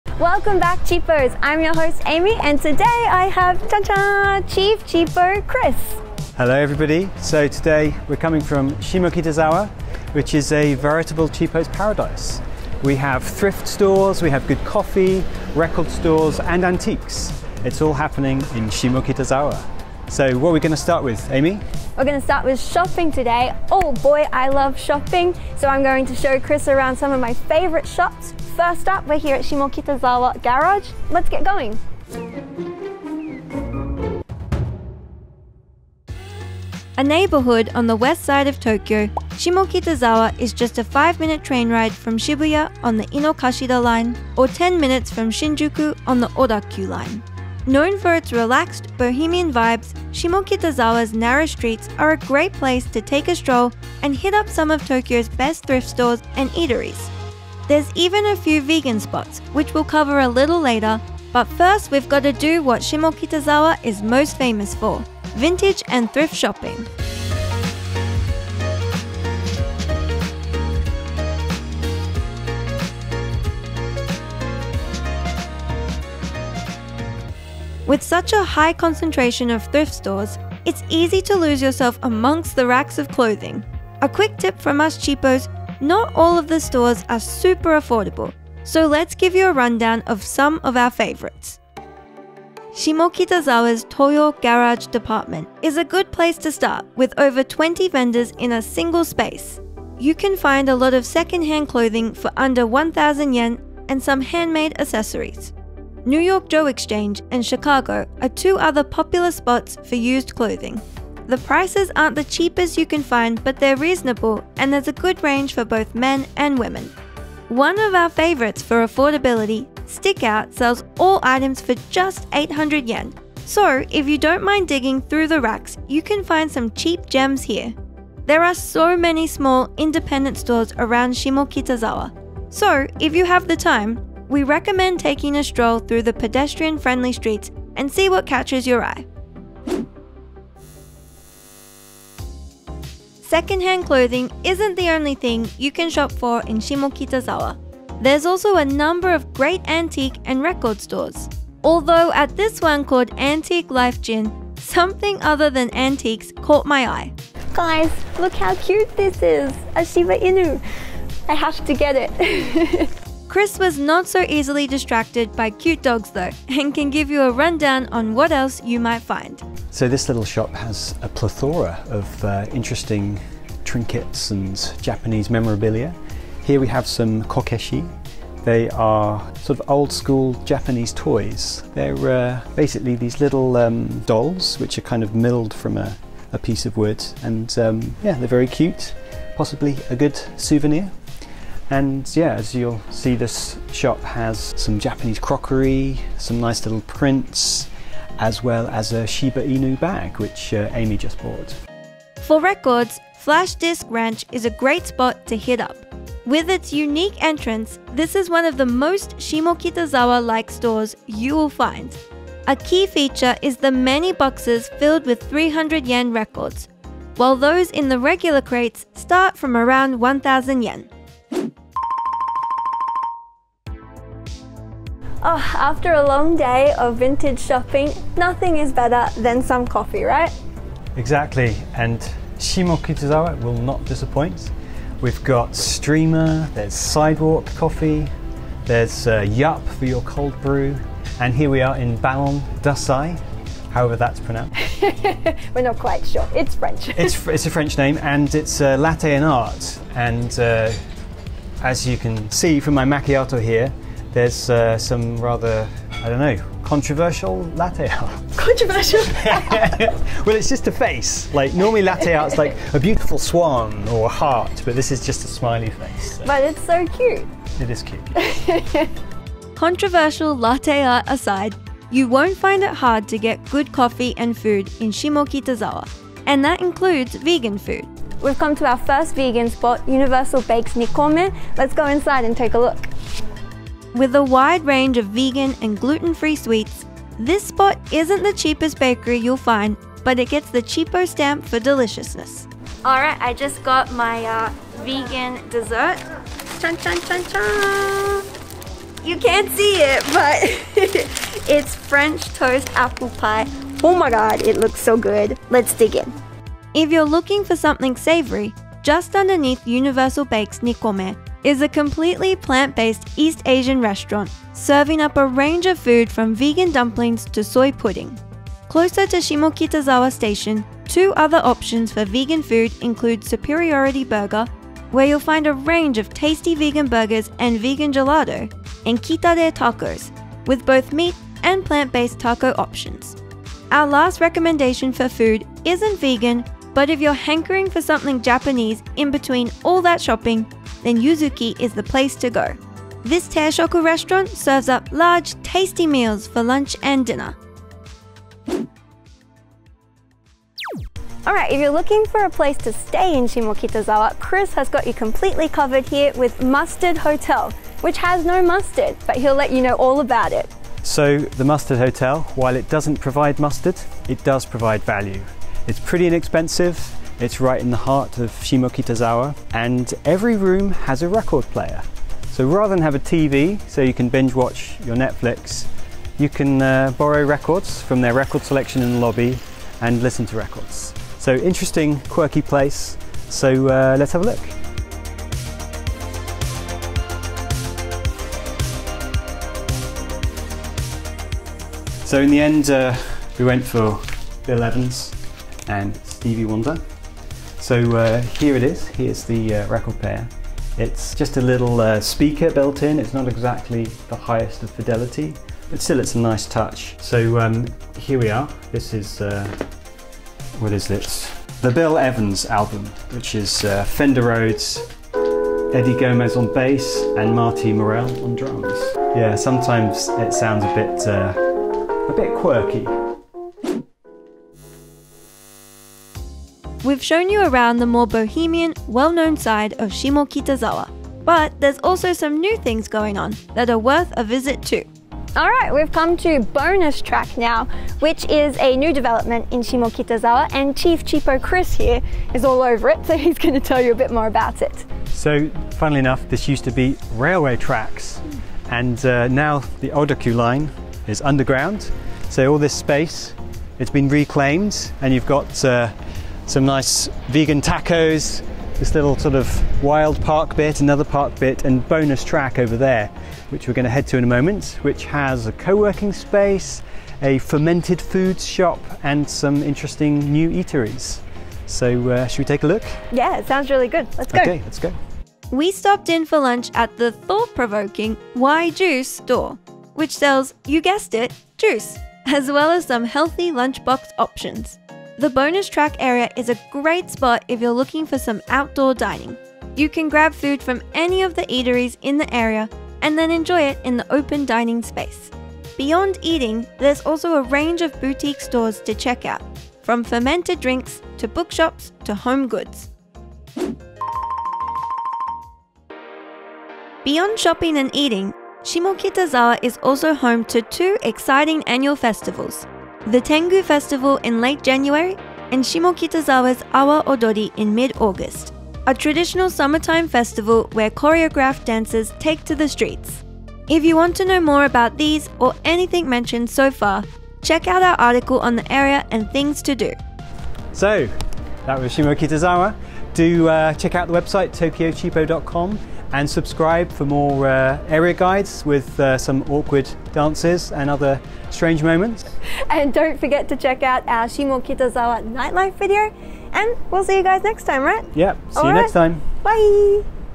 Shimokitazawa Guide - Vintage Shopping, Coffee, Traditional Festivals & more!
Shimokitazawa -- often shortened to Shimokita -- is a trendy, bohemian neighborhood just a few train stops from central Tokyo. The narrow streets here are stuffed with vintage clothing shops, quirky cafes, healthy and vegan food, live houses, indie fashion outlets, and artist studios. It's an oasis for creative types, as well as cheapos looking to snag some bargains.

0:00 Introduction
0:35 Thrift & Vintage Shopping
2:21 Antique & Vinyl Shopping
3:43 Coffee and Vegan Food
5:50 Mustard Hotel
7:05 Bonus Track
8:26 Shimokitazawa Festivals

★★★ Credits ★★★
Intro sequence by the most talented Peter Laugesen